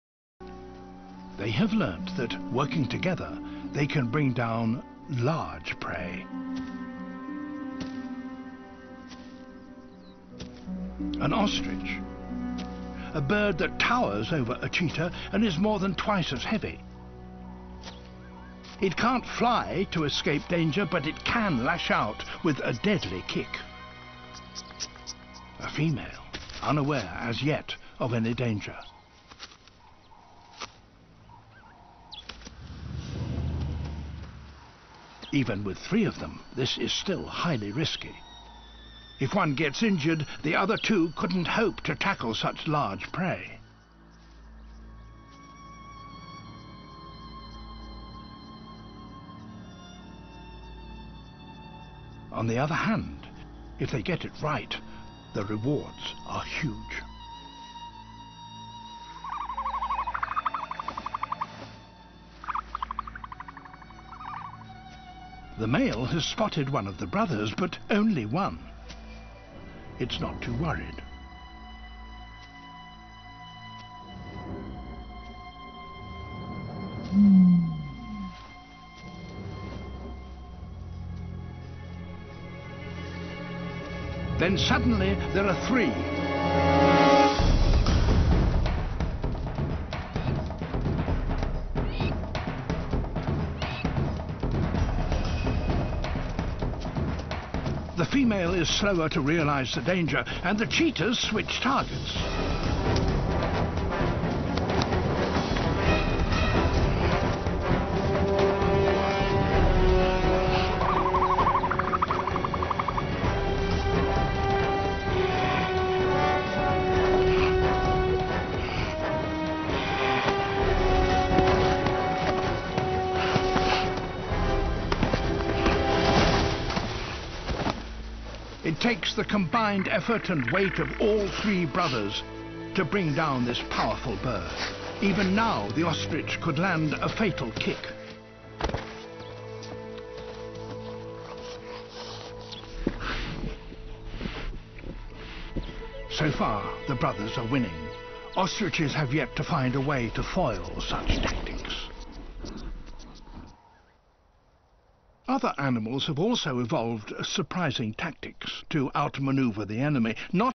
Wild Animal Hunting- cheetahs hunt ostrich
This short video shows you how cheetahs hunt one ostrich.